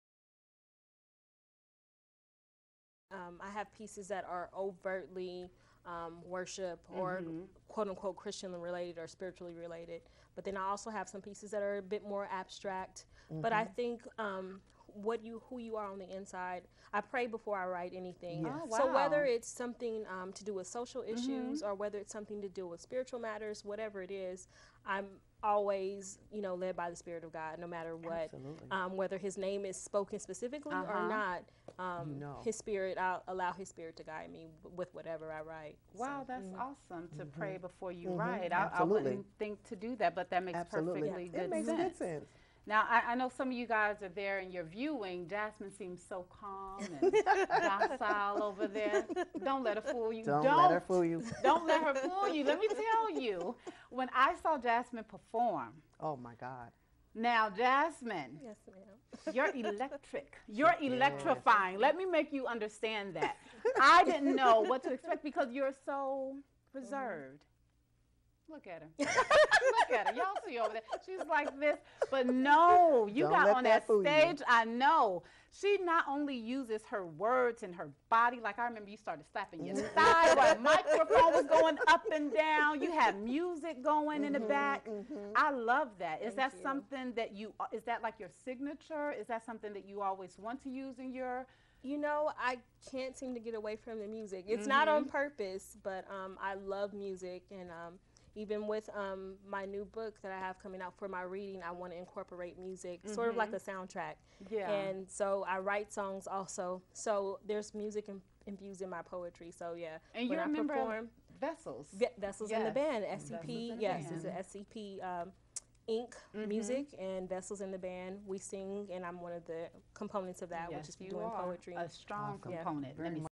Christian Poet Jasmine N. Colbert
Meet Jasmine N. Colbert. Author of "Two Sides of the Same Hunger"
Arroyo CH 32 | Tuesday @ 8:30am/pm PST